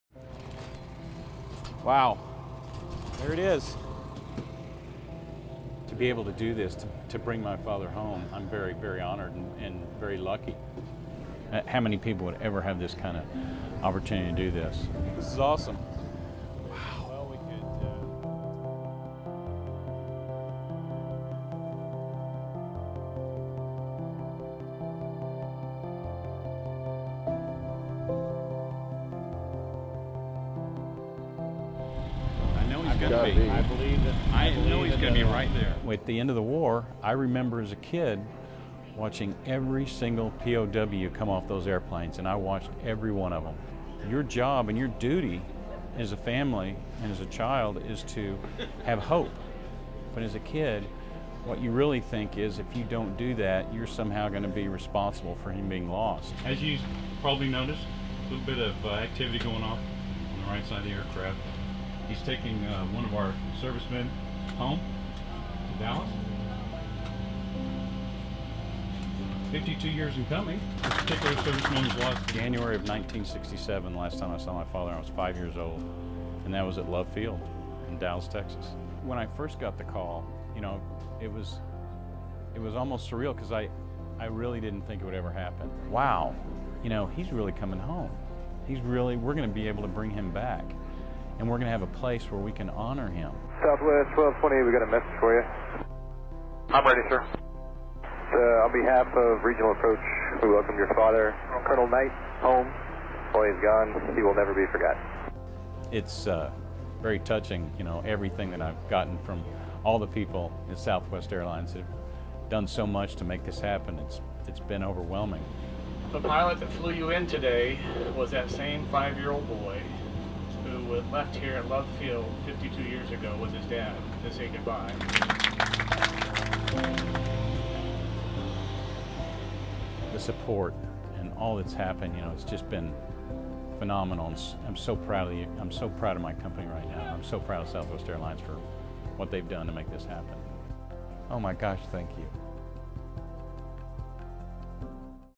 Southwest Captain Brings His Dad Home
Southwest Airlines Captain Bryan Knight last saw his father, Col. Roy A Knight, at Dallas Love Field Airport when he was five years old. Col. Knight deployed to serve in the Vietnam War in 1967, where he was shot down and went missing in action. 52 years later, his body was identified. Captain Knight, also a USAF veteran, flew his father home to Dallas for the final time.